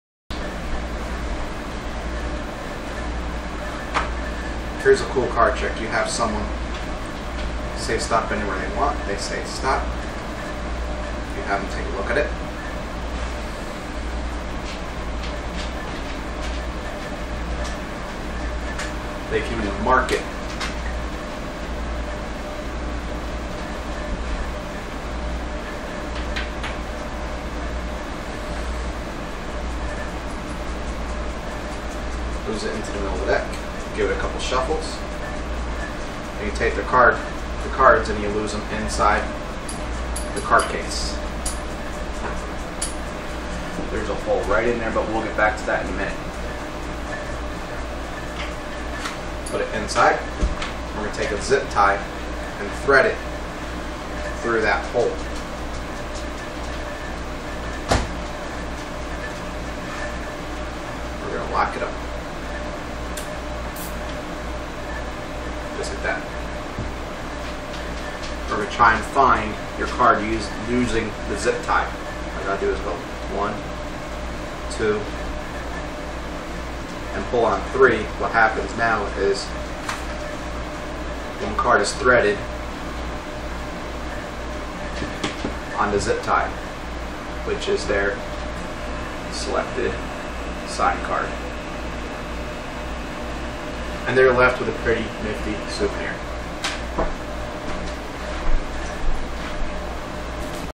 impaled
Spectators signed card appears on a ziptie